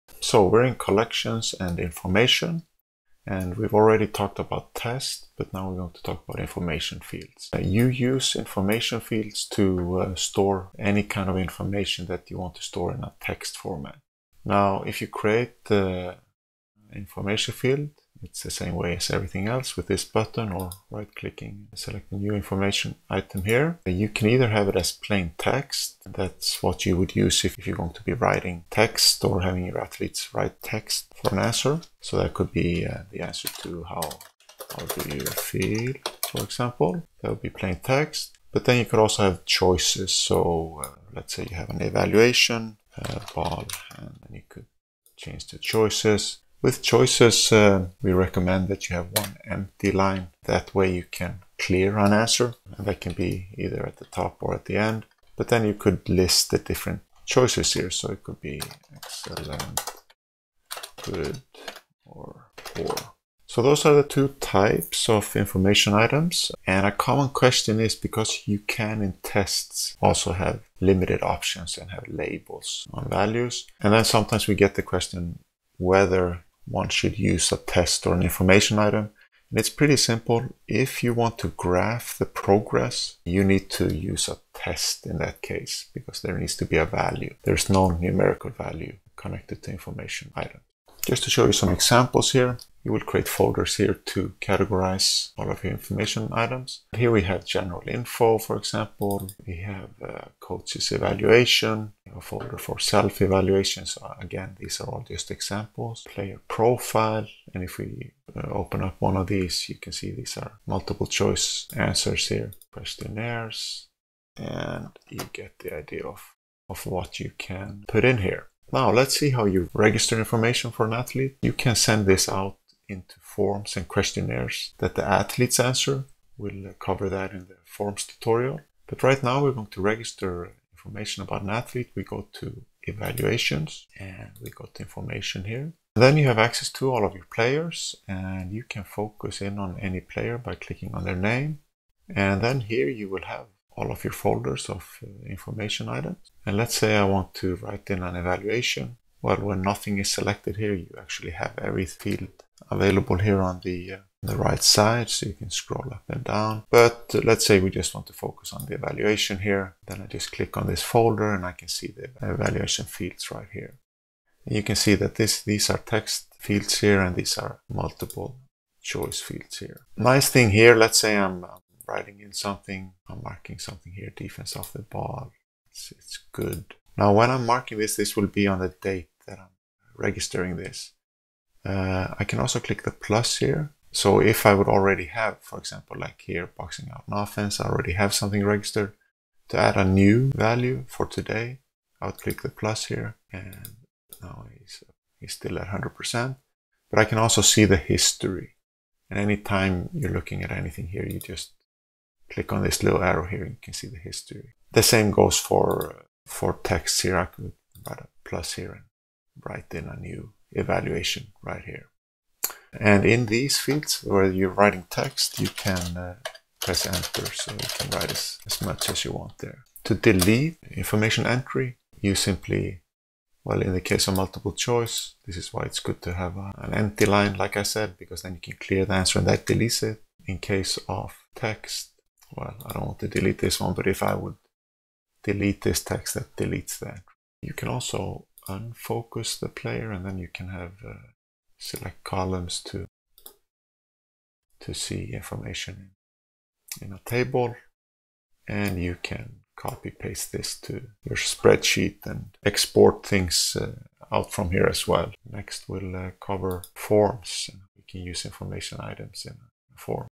8.2 Information Collection | XPS Courses & Tutorials
- Create a new information field 0:07
- Information fields examples 1:31
- View and register information for an athlete 2:07
- See history of information entries 3:16
- Use Enter to add new text lines / Clear field of text to delete the entry 3:32
- Use columns to view and export information from multiple athletes 3:57